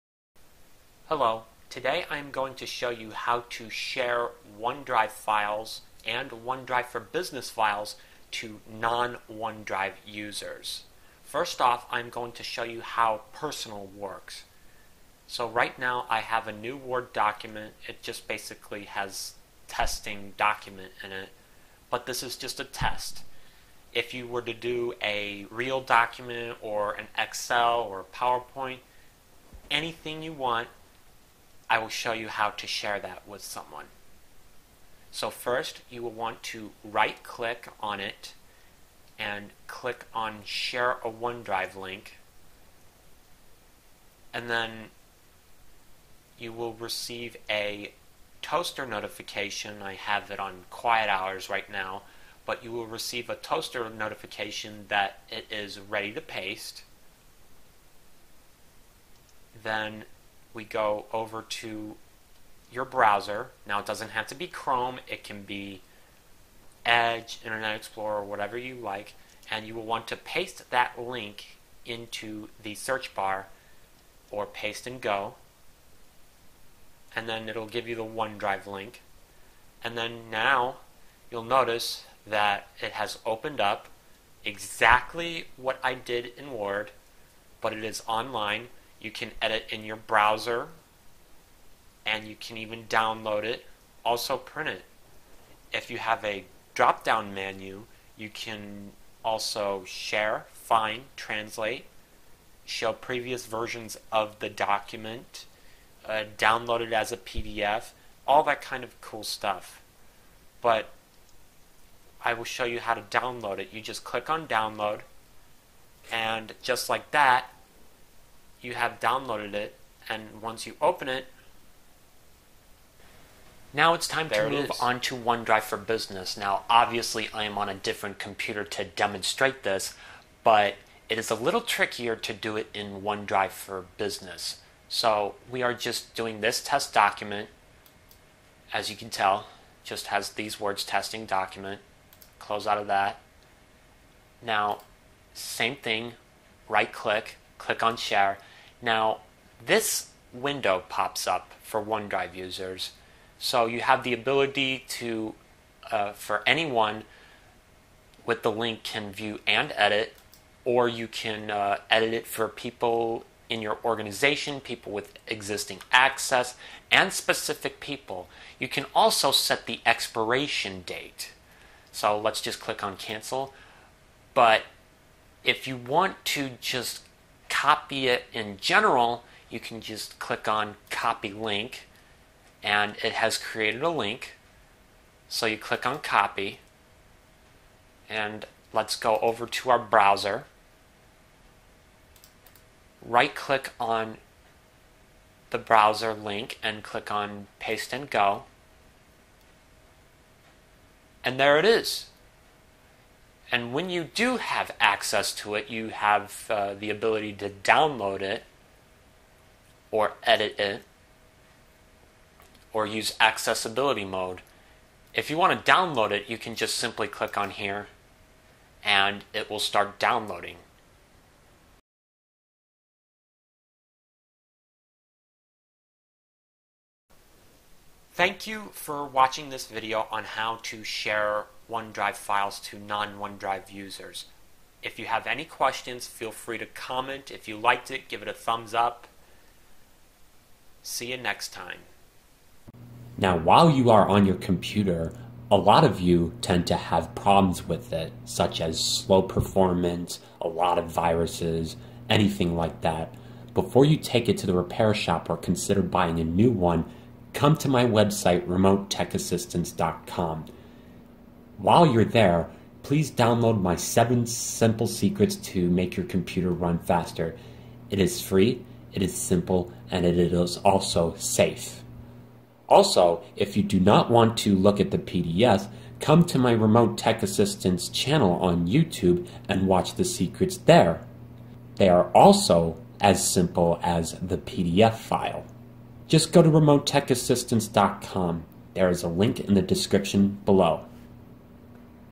How to Share OneDrive Links With Non OneDrive Users
Many of you do not have the same cloud storage as I do. You usually have Dropbox, Google Drive, or no cloud storage at all. With this tutorial, I will show you how to share OneDrive links with other people who do not use OneDrive at all. OneDrive unmistakably makes things so easy, that anyone can do it!